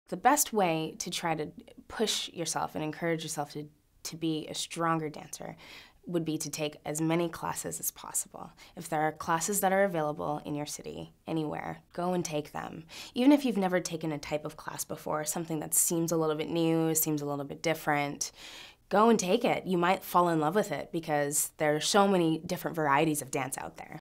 Dancer: Skills to Develop - Rachael McLaren Career Girls Role Model
Rachael McLaren, dancer with Alvin Ailey American Dance Theater, shares valuable dance career guidance and life advice with girls. Learn how to become a professional dancer and watch her full interview